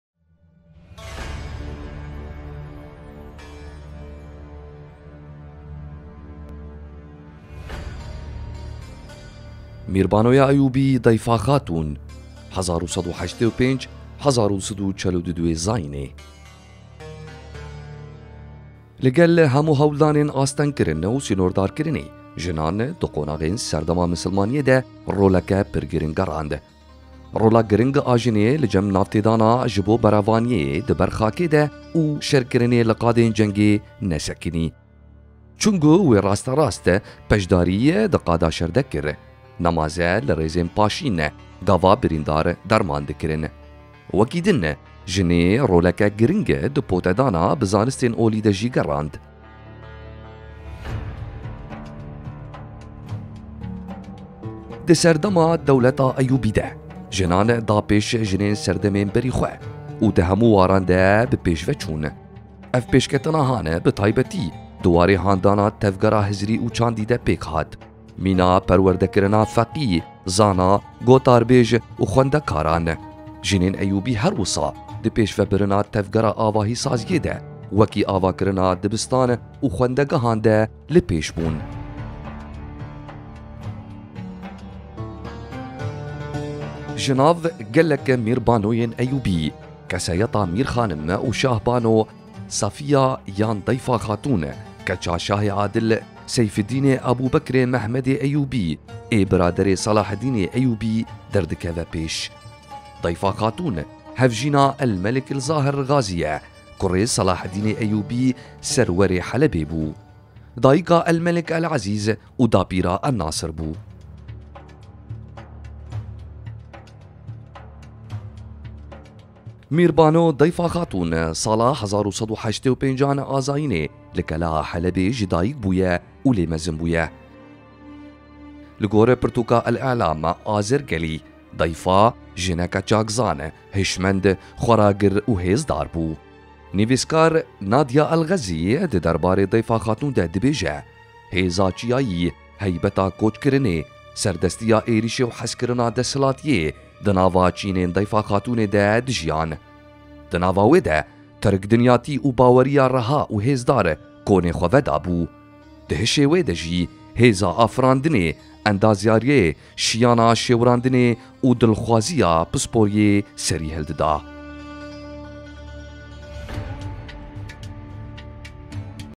ضيفة خاتون ودور المرأة الأيوبية
مقطع قصير عن ضيفة خاتون ودور المرأة الأيوبية في أقل من 3 دقائق